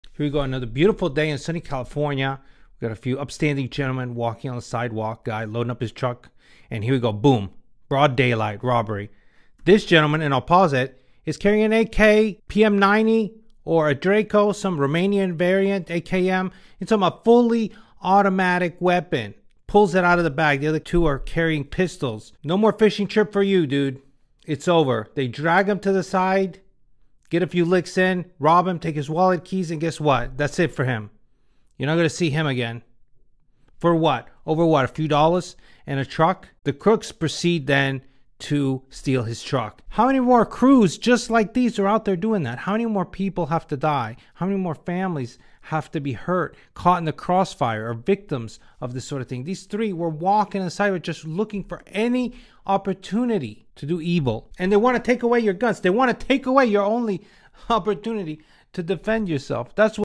Broad Daylight Robbery
Everyones worst nightmare: being attacked, and robbed right in front of your home in brad daylight.